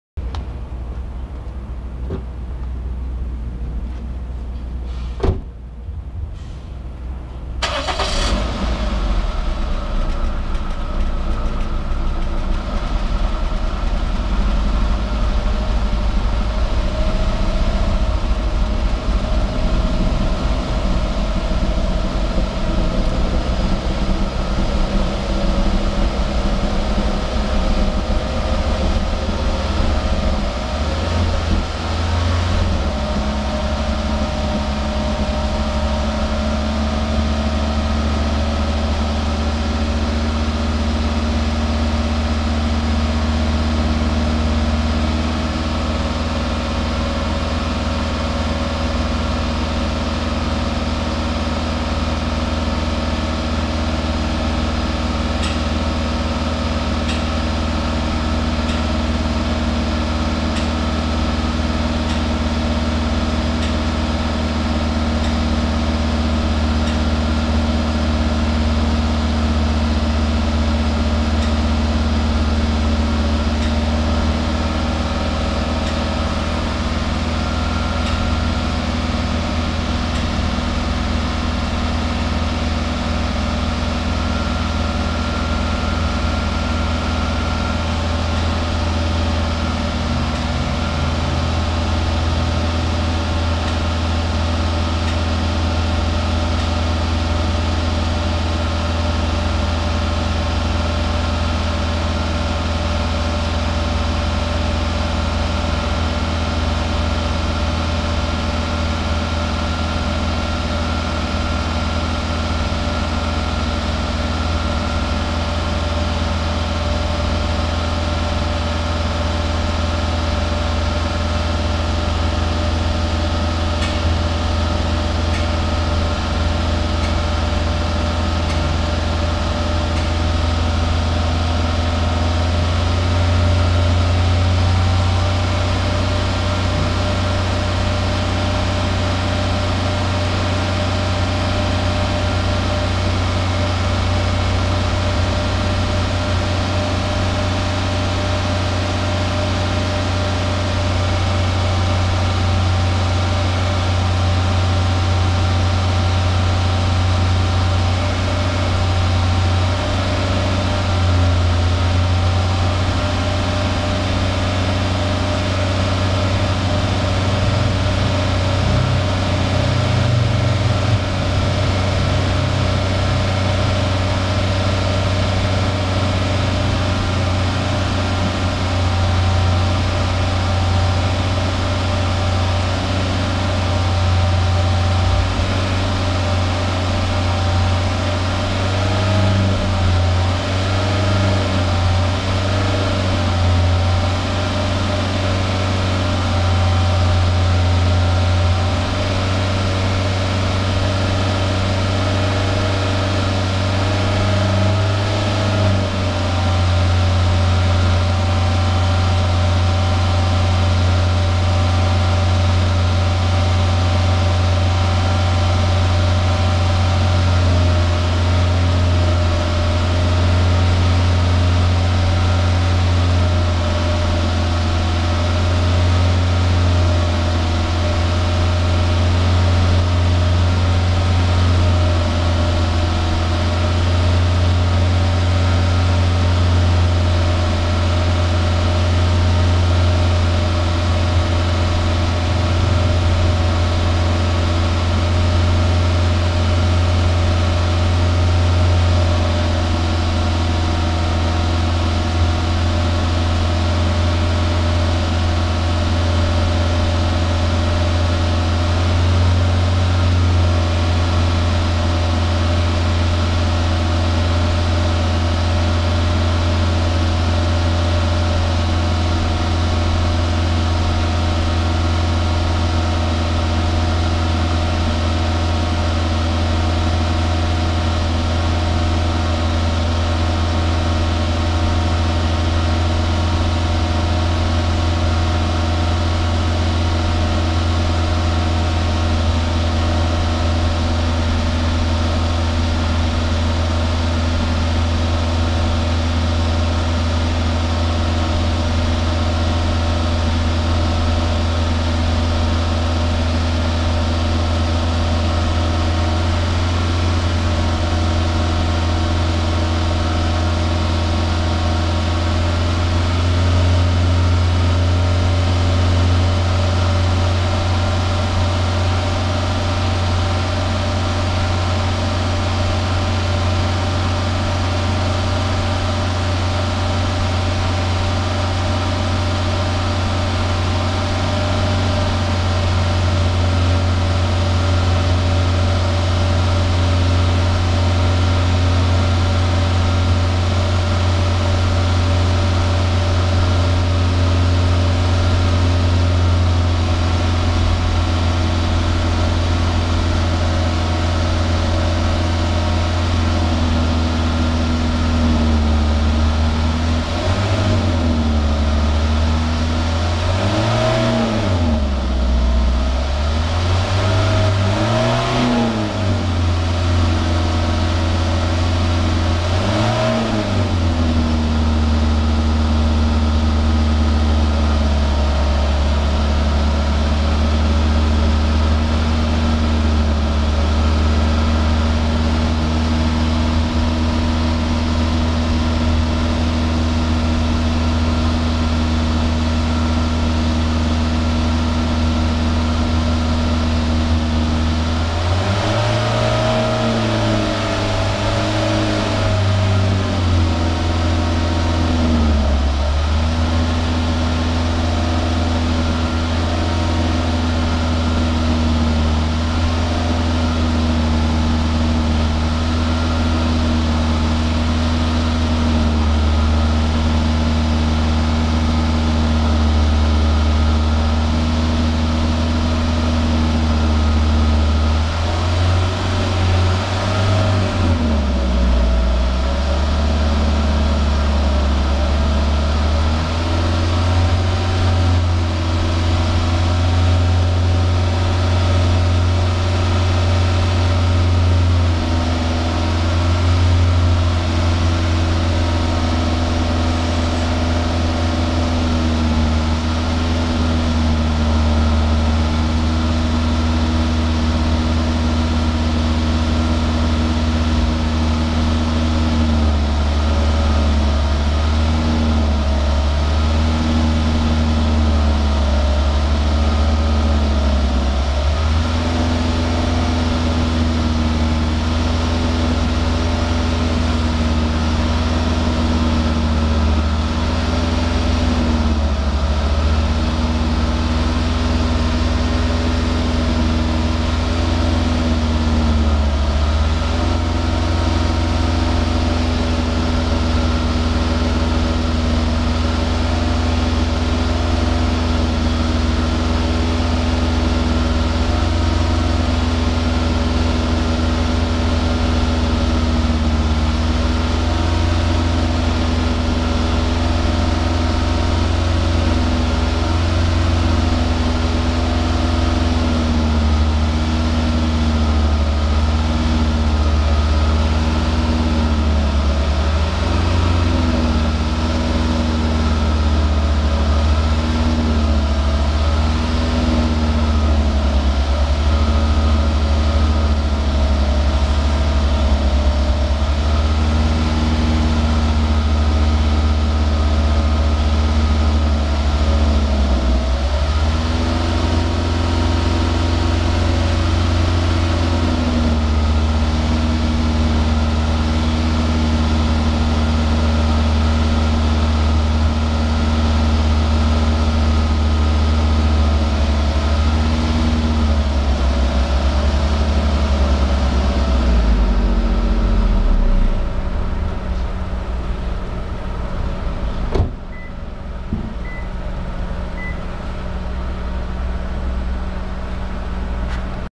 ワコーズ RECS、施工後の愛車。エンジンをふかしてみたら、いつまでも白煙が(￣Д￣;;
2017年12月8日（金曜日）

約６ヶ月半ぶりの、
自分の愛車へのワコーズRECS施工。
（1300CC　エンジン型式 2SZ-FE）

私が購入した注入キットは、あまり出来が良いものではなく、
薬剤の注入にあまりにも時間がかかりすぎるので、
（完全に一時間以上かかりましたよ、その時は）
今回は注入キットの「空気穴」をガムテープでふさぐことで、
大幅に薬剤の注入速度をスピードアップさせました。
今回は、おそらく15分ぐらいで140CCの注入が出来たと思います。

さて、薬剤の注入が終わったので、エンジンをかけて
空ぶかしをしてみると・・・(;ﾟロﾟ)
うわー、凄まじい白煙！
しかも、いつまでもいつまでも出続けます(￣Д￣;;　げげー

あまりにも長い間白煙が止まらないので、いったんエンジンを切り、
周辺の空いている道で、エンジンの回転数を回し気味で
ひとっ走りして来ました。
そうすると、やっと白煙が収まりました。

前回は、注入速度があまりにも遅すぎたので、
汚れを落とす効果が十分でなかったのかも知れません。

今回の施工後の方が、確実にエンジンのフィーリングが
良くなっているなと実感できました。